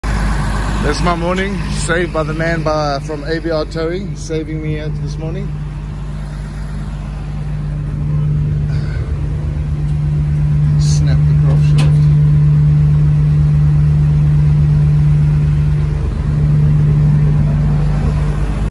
The Cummins 6bt snaps the propshaft yolk and the universal joint
The Cummins 6bt snaps the prop shaft yolk and universal